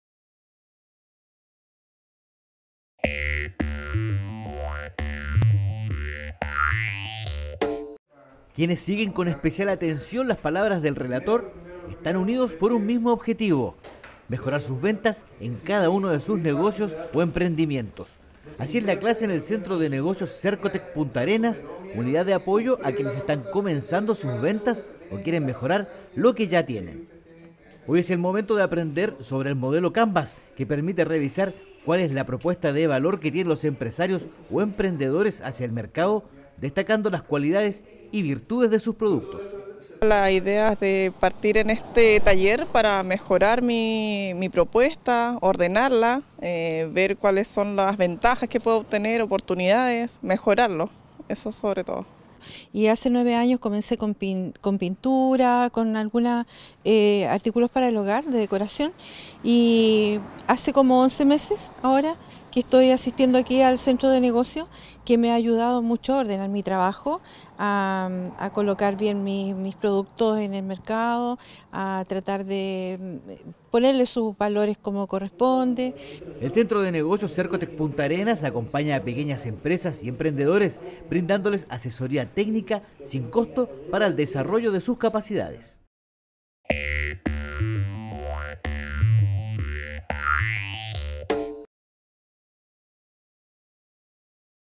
Emprendedores se capacitan en modelo de negocios Canvas
El Centro de Negocios Sercotec Punta Arenas acompaña a pequeñas empresas y emprendedores brindándoles asesoría técnica y sin costo para el desarrollo de sus capacidades.